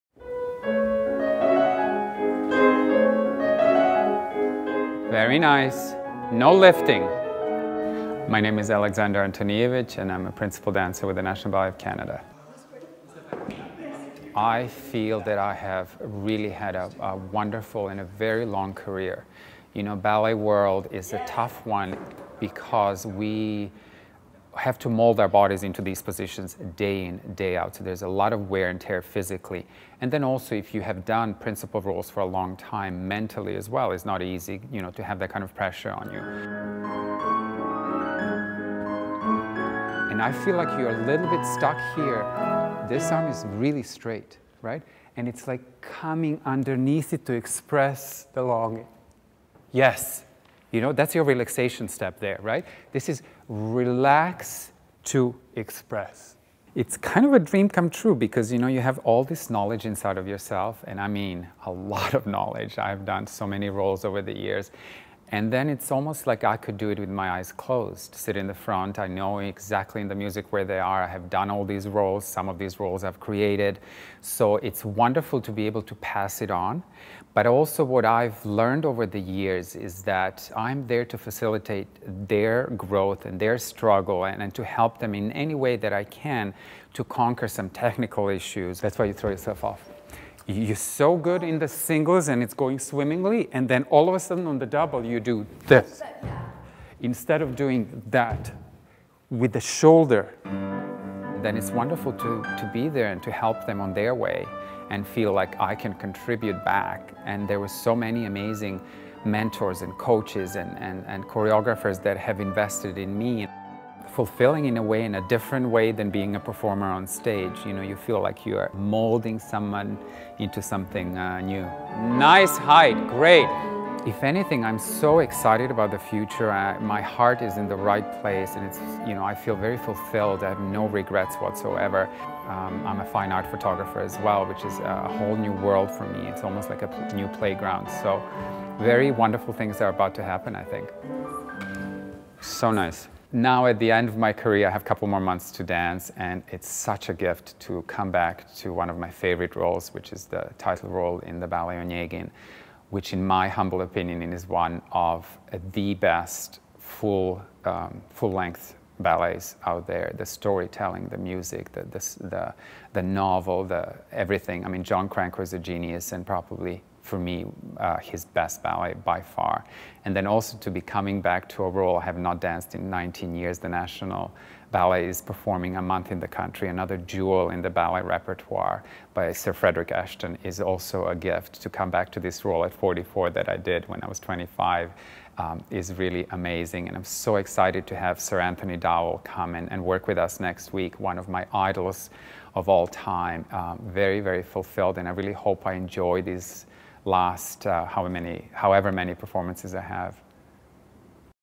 Aleksandar Antonijevic set to retire from National Ballet
Withing a few months, National Ballet of Canada Principal Dancer Aleksandar Antonijevic, will retire from dancing to focus on handing down his years of experience to others through coaching and mentoring.